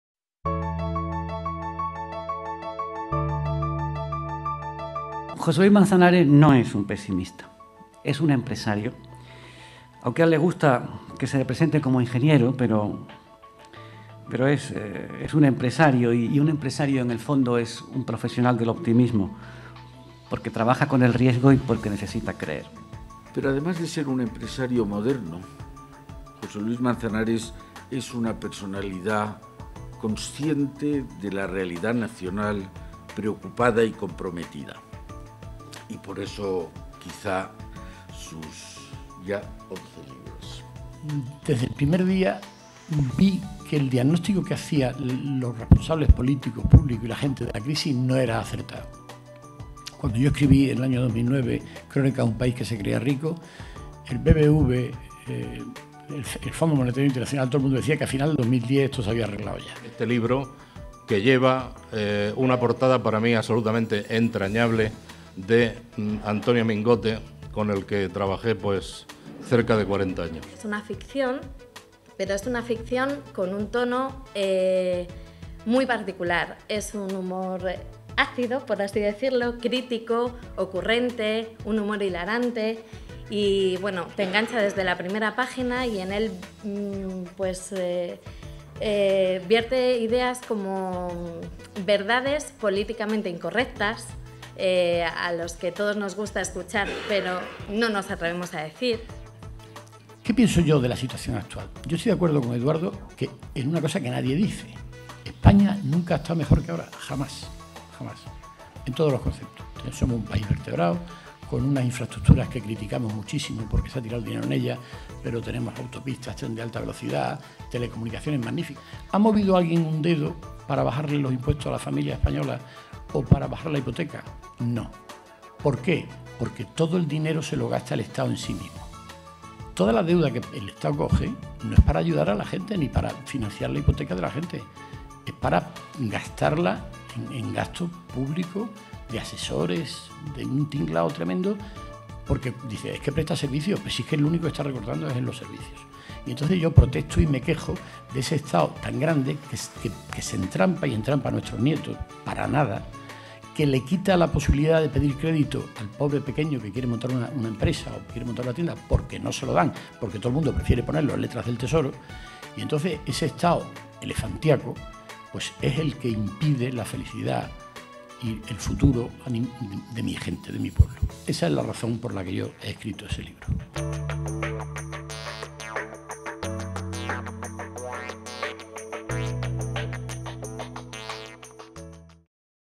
Presentación del libro Crónicas de un país que no quería ser pobre en Madrid - José Luis Manzanares
«España nunca ha estado mejor que ahora», «la recuperación depende de nosotros», este es el mensaje que José Luis Manzanares transmitió durante la presentación de Crónicas de un país que no quería ser pobre.

Una obra que supone un toque de atención a la clase política y a la ciudadanía para construir un país mejor. Aquí el vídeo resumen del acto de presentación en la Asociación de la Prensa de Madrid.